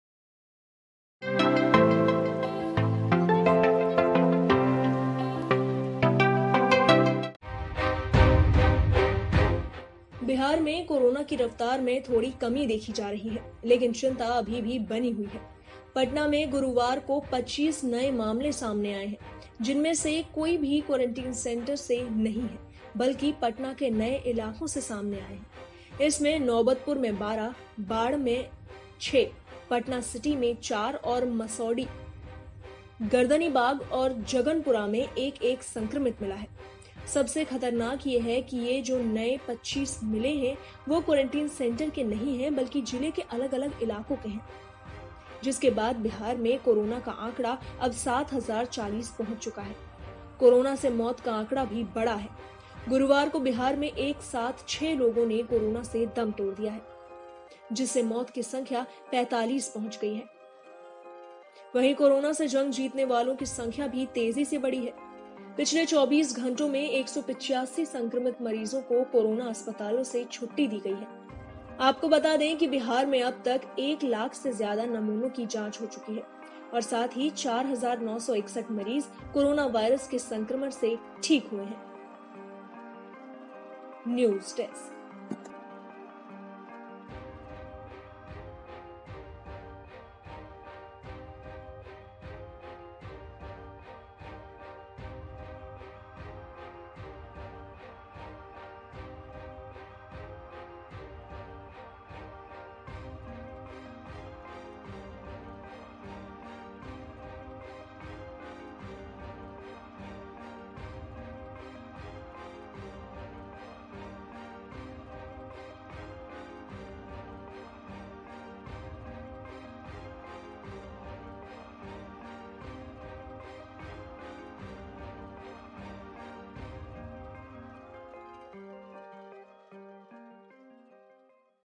Bihar में Corona के 25 नए मामले आए सामने, आंकड़ा पहुंचा 7000 के पार
बिहार में कोरोना की रफ़्तार में थोड़ी कमी देखी जा रही है लेकिन चिंता अभी भी बनी हुई है... पटना में गुरुवार को 25 नए मामले सामने आए हैं. जिसमें से कोई भी क्वारंटाइन सेंटर से नहीं है, बल्कि पटना के नए इलाकों से सामने आएं हैं.